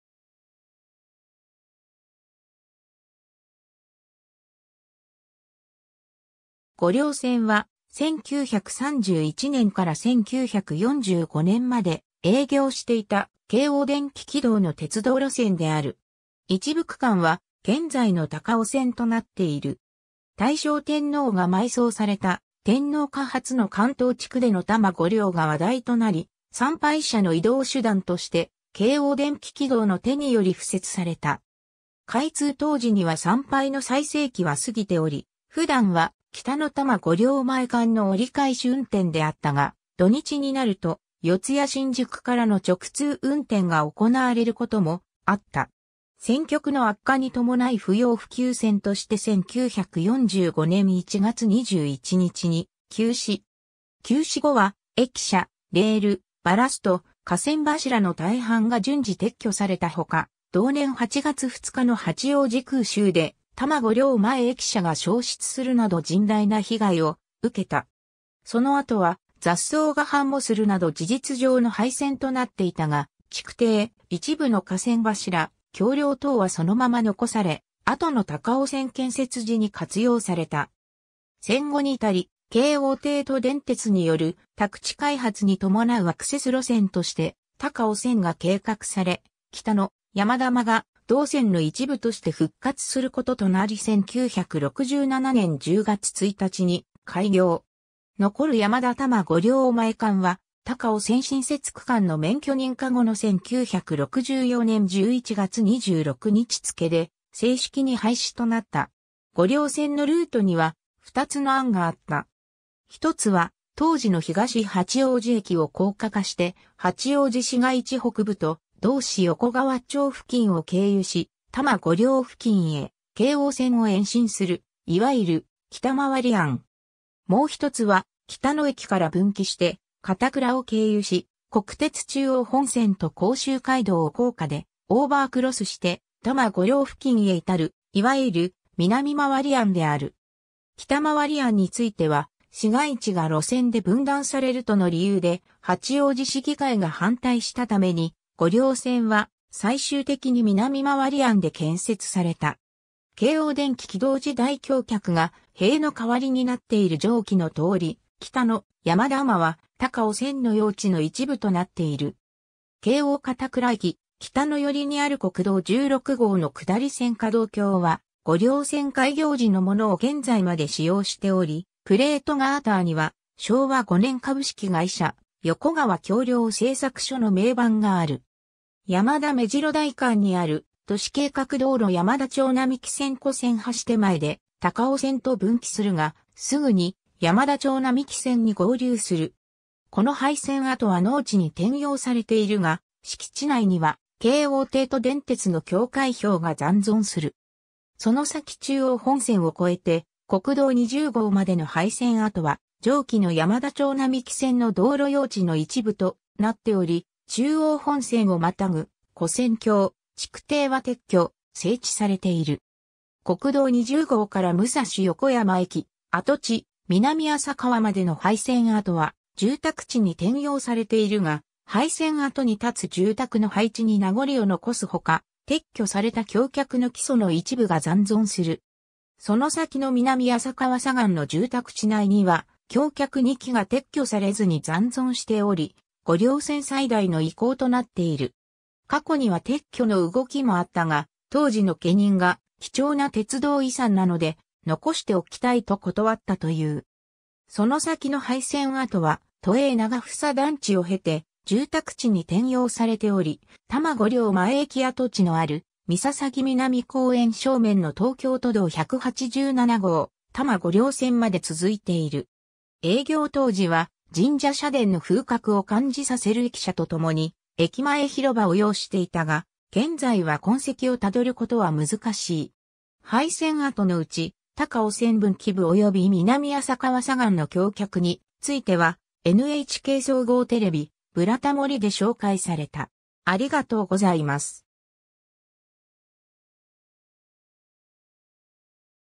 京王御陵線
関東地方の鉄道路線_(廃止)
御陵線（ごりょうせん）は、1931年（昭和6年）から1945年（昭和20年）まで営業していた京王電気軌道（→東京急行電鉄→京王帝都電鉄〈現・京王電鉄〉）の鉄道路線である。
一部区間は現在の高尾線となっている。
 大正天皇が埋葬された天皇家初の関東地区での多摩御陵が話題となり、参拝者の移動手段として京王電気軌道の手により敷設された。
開通当時には参拝の最盛期は過ぎており、普段は北野 - 多摩御陵前間の折り返し運転であったが、土日になると四谷新宿（当時）からの直通運転が行われることもあった。
 戦局の悪化に伴い不要不急線として1945年（昭和20年）1月21日に休止。
休止後は駅舎、レール、バラスト、架線柱の大半が順次撤去された他、同年8月2日の八王子空襲で多摩御陵前駅舎が焼失するなど甚大な被害を受けた。
 その跡は雑草が繁茂するなど事実上の廃線となっていたが、築堤、一部の架線柱、橋梁等はそのまま残され、後の高尾線建設時に活用された。
 戦後に至り、京王帝都電鉄（当時）による宅地開発に伴うアクセス路線として高尾線が計画され、北野 - 山田間が同線の一部として復活することとなり1967年（昭和42年）10月1日に開業。
残る山田 - 多摩御陵前間は高尾線新設区間（山田 - 高尾山口間）の免許認可後の1964年（昭和39年）11月26日付で正式に廃止となった。
 御陵線のルートには、二つの案があった。
一つは当時の東八王子駅（現・京王八王子駅）を高架化して、八王子市街地北部と同市横川町付近を経由し、多摩御陵付近へ京王線を延伸するいわゆる北回り案。
もう一つは北野駅から分岐して片倉を経由し、国鉄中央本線と甲州街道を高架でオーバークロスして、多摩御陵付近へ至るいわゆる南回り案である。
北回り案については、市街地が路線で分断されるとの理由で八王子市議会が反対したために、御陵線は最終的に南回り案で建設された。
 京王電気軌道時代 橋脚が塀の代わりになっている 上記の通り、北野 - 山田間は高尾線の用地の一部となっている。
京王片倉駅北野寄りにある国道16号の下り線架道橋は、御陵線開業時のものを現在まで使用しており、プレートガーターには「昭和五年 株式會社横河橋梁製作所」の銘板がある。
 山田 - めじろ台間にある都市計画道路山田町並木線跨線橋手前で高尾線と分岐するが、すぐに山田町並木線に合流する。
この廃線跡は農地に転用されているが、敷地内には京王帝都電鉄（当時）の境界標が残存する。
その先中央本線を越えて国道20号（甲州街道）までの廃線跡は、上記の山田町並木線の道路用地の一部となっており、中央本線を跨ぐ跨線橋・築堤は撤去・整地されている。
 国道20号から武蔵横山駅跡地、南浅川までの廃線跡は住宅地に転用されているが、廃線跡に建つ住宅の配置に名残を残す他、撤去された橋脚の基礎の一部が残存する。
その先の南浅川左岸の住宅地内には、橋脚2基が撤去されずに残存しており、御陵線最大の遺構となっている。
過去には撤去の動きもあったが、当時の（橋脚が建つ敷地の）家人が「貴重な鉄道遺産なので、残しておきたい」と断ったという。
 その先の廃線跡は、都営長房団地を経て住宅地に転用されており、多摩御陵前駅跡地のある陵南公園正面の東京都道187号多摩御陵線まで続いている。
営業当時は神社社殿の風格を感じさせる駅舎と共に駅前広場を擁していたが、現在は痕跡を辿ることは難しい。
 廃線跡のうち、高尾線分岐部及び南浅川左岸の橋脚については、NHK総合テレビ『ブラタモリ』（2016年9月17日放送・「#47 高尾山」回）で紹介された。